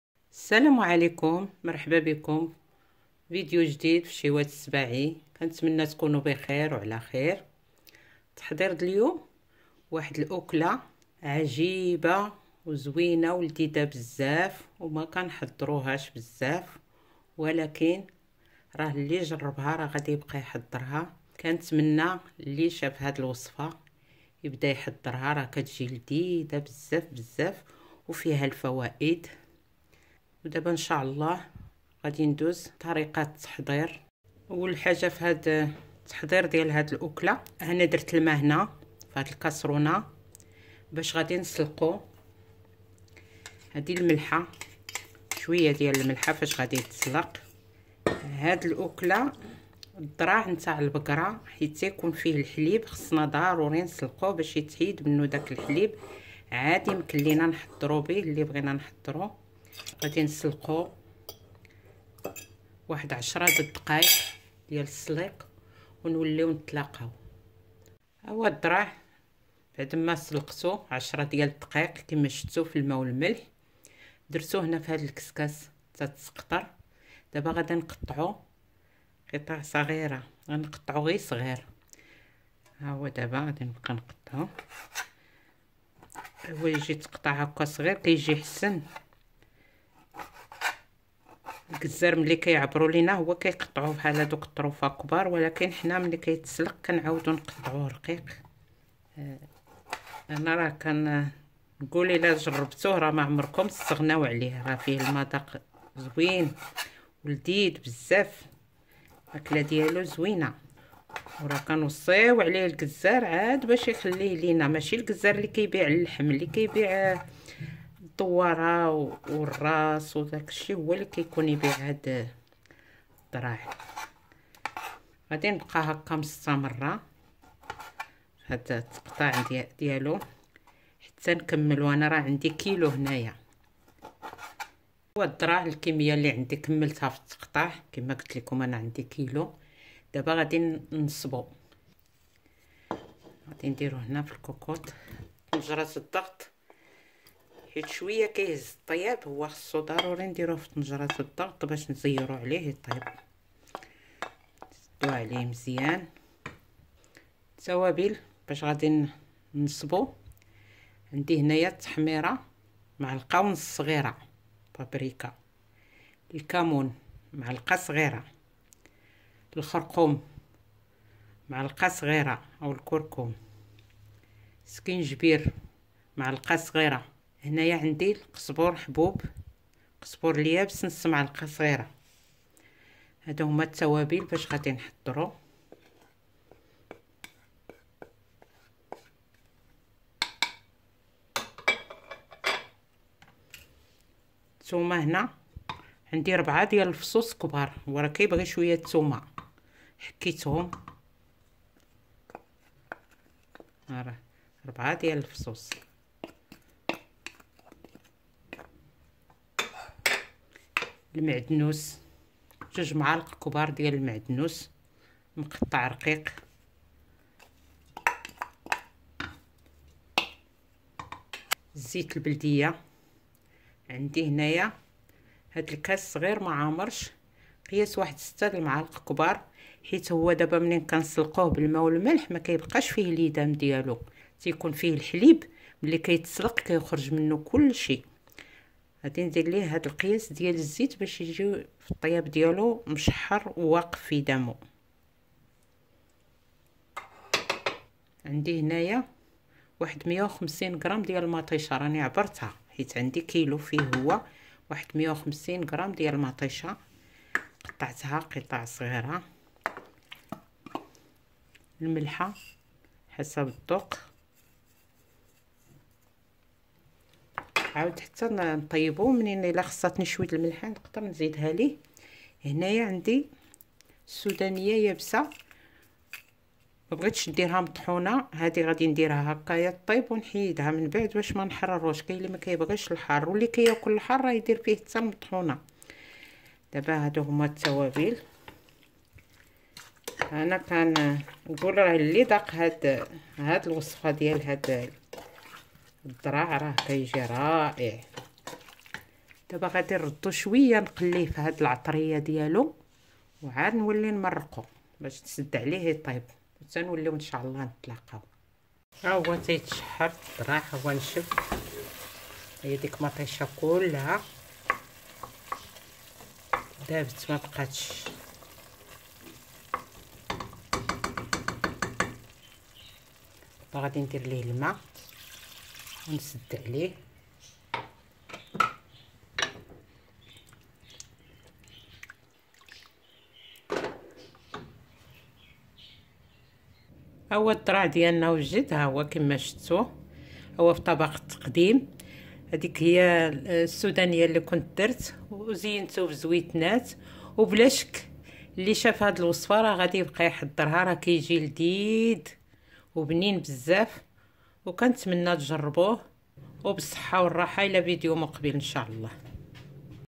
وصفة لتحضير ضرع البقري مغذية ولذيذة. سهلة التحضير Pis de vache
المكونات:
1 كغ ضرع البقر
ملعقة صغيرة و نصف بابريكا
1 ملعقة صغيرة كمون
ملعقة صغيرة كركم
ملعقة صغيرة زنجبيل
نصف ملعقة صغيرة بذور كزبرة مرحية
4 فصوص ثوم
4 ملاعق كبيرة بقدونس
6 ملاعق كبيرة زيت زيتون
150 غ طماطم
ملح
فلفل حار أحمر
ضرع_البقر#